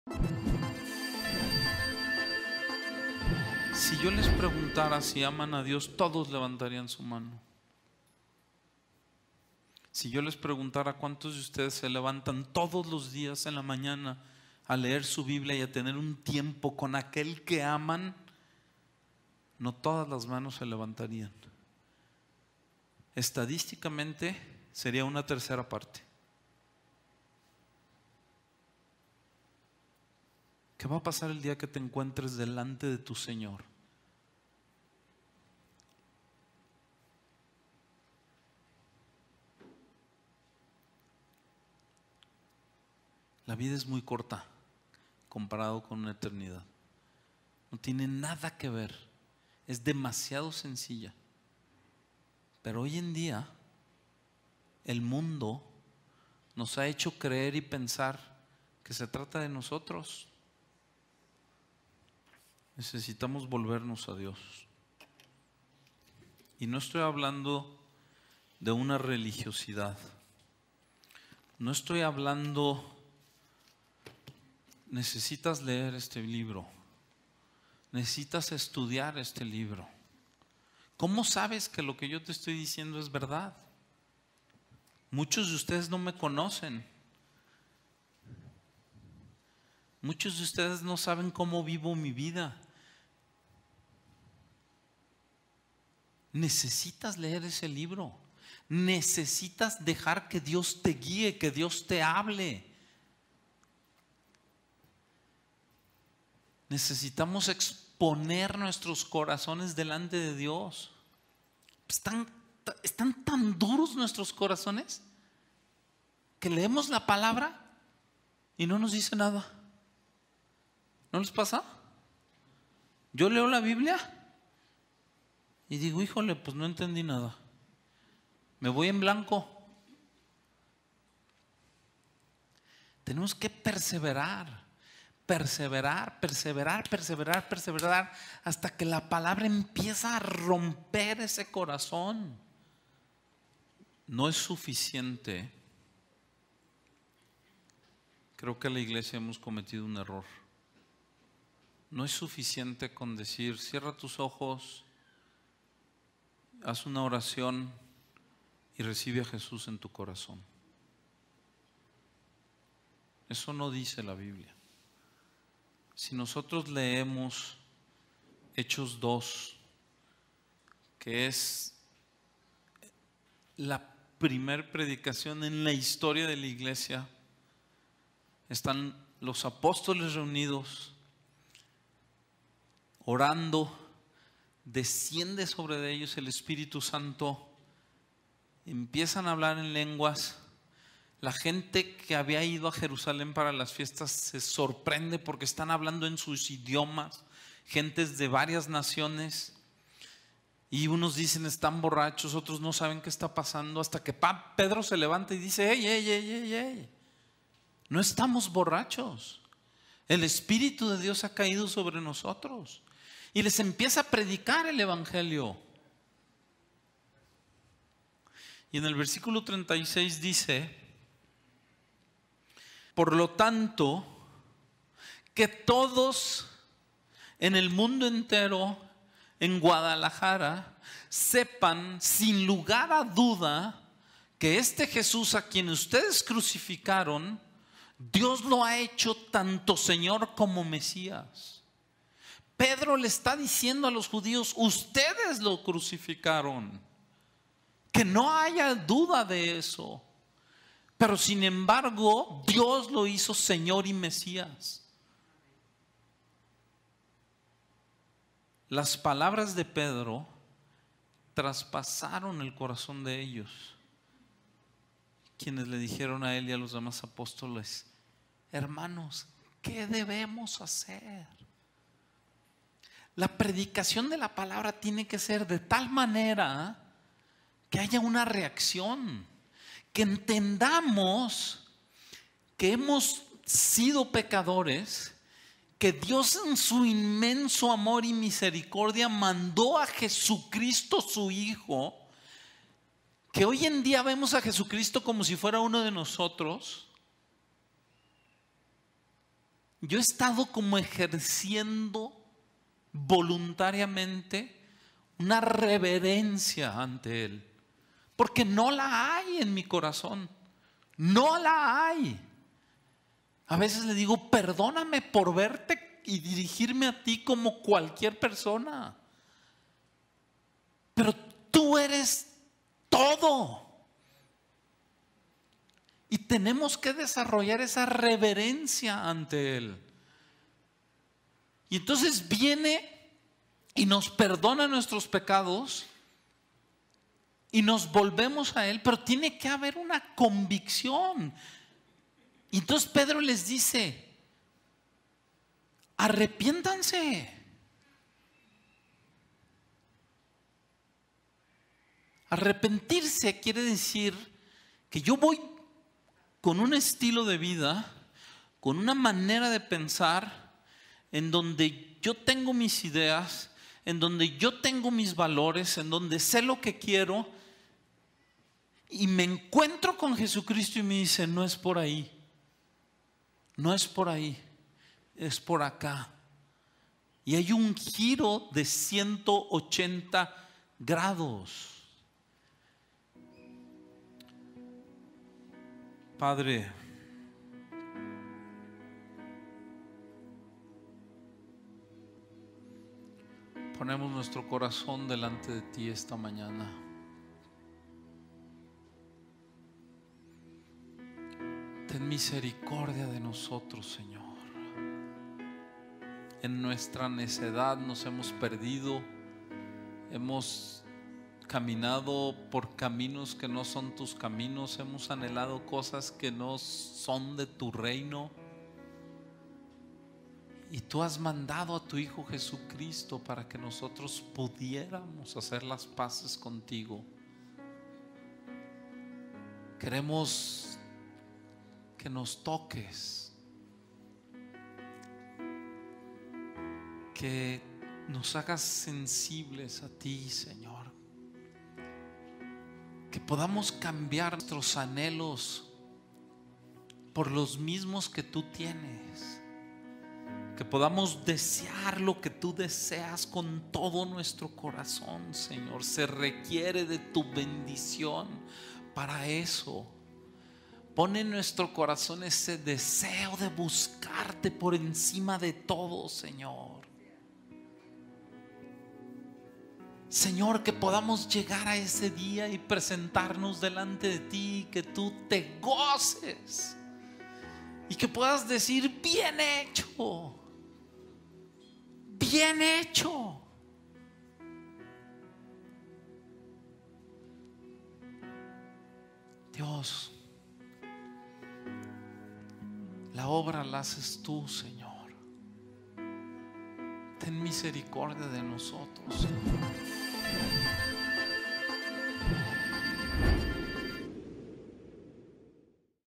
Juan Pablo Mackissack - Jesús - La Iglesia - Enlace TV
Juan Pablo Mackissack nos comparte acerca de que la vida, contrario a lo que el mundo nos enseña, no se trata de nosotros. Todo se trata de Dios.

 Un mensaje práctico y fresco con vulnerabilidad por parte de nuestros Pastores en donde podrás entender la palabra De Dios y aplicarla a tu vida.

Puedes ver más episodios de este programa en Enlace+

Descarga nuestra app Enlace+ en
App store